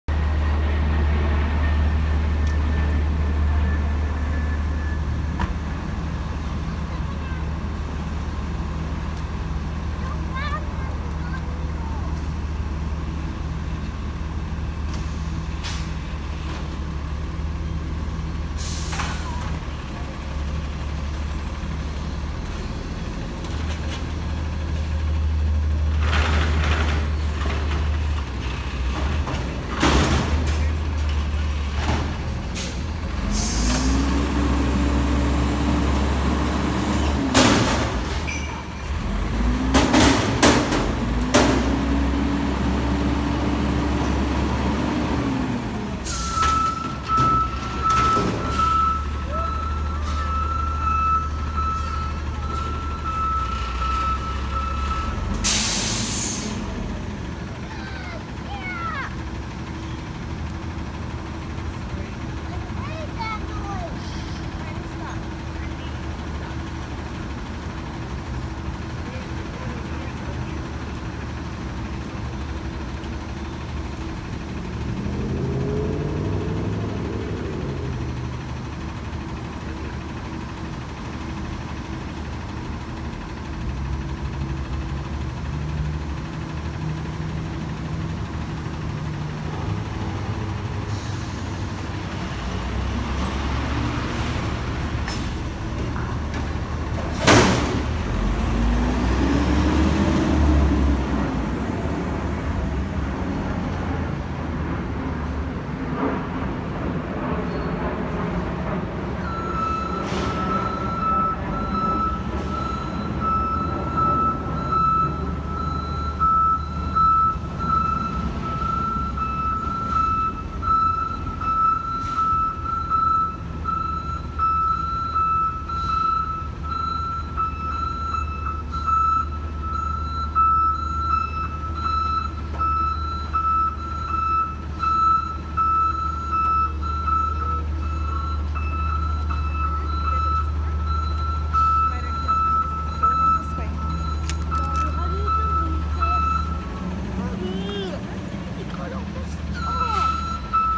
Green Team of San Jose Trash Trucks
Front loader green team garbage truck picks up dumpster in downtown San Jose. July 18, 2019